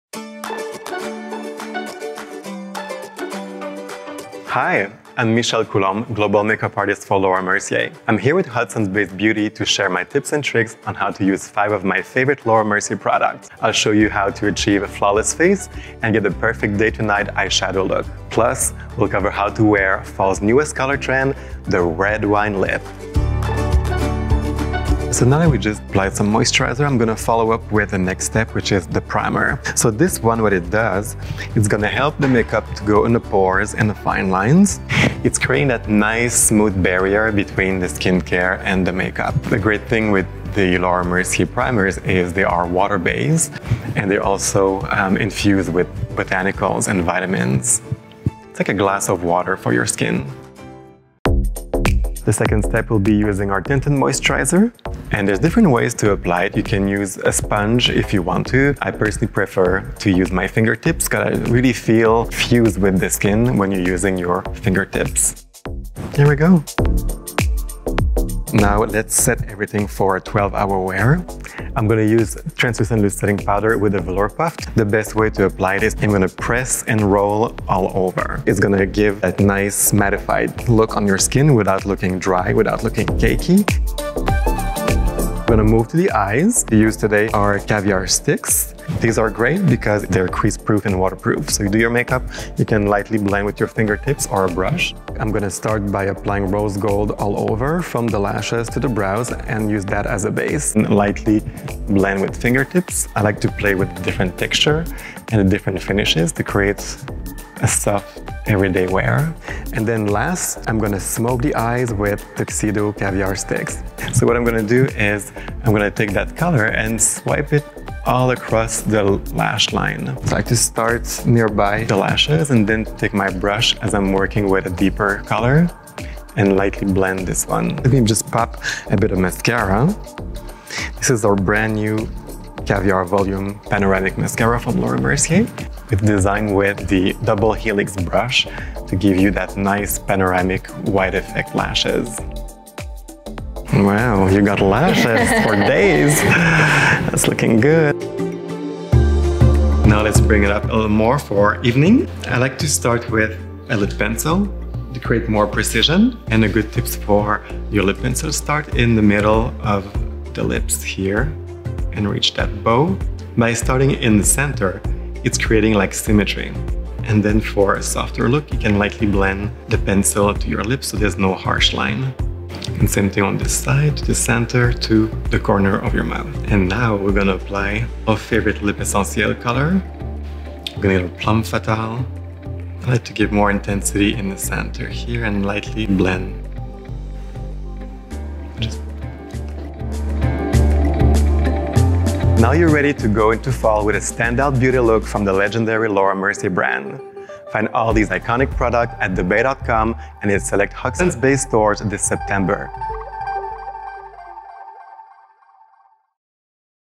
5 Steps for a Flawless Face | Laura Mercier
Laura Mercier Cosmetics Global Makeup Artist Michel Coluombe will show you how to achieve a flawless look in just 5 steps using products from Laura Mercier & Hudson's Bay.

Follow @hudsonsbay for more